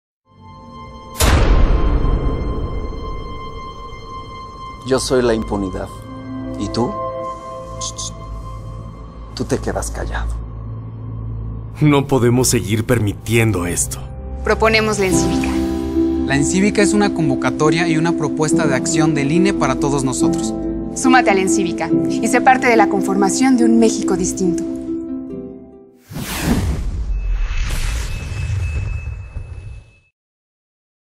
Impunidad | ENCCÍVICA
La estrategia Nacional de Cultura Cívica (ENCCÍVICA) es el resultado de una deliberación amplia de múltiples actores (públicos, privados y sociales) que participaron en la discusión del diagnóstico que le da sustento al problema de la política pública, como en la integración de propuestas para precisar acciones que contribuyan al fortalecimiento de la cultura democrática de México.